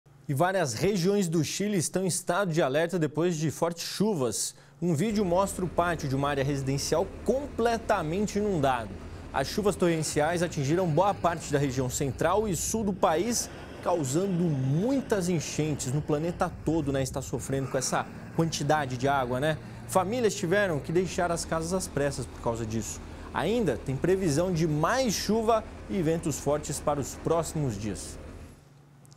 Fortes chuvas atingem várias regiões do Chile
O Chile segue em estado de alerta e há previsão de mais chuva e ventos fortes para os próximos dias. A tempestade atingiu boa parte da região central e sul do país.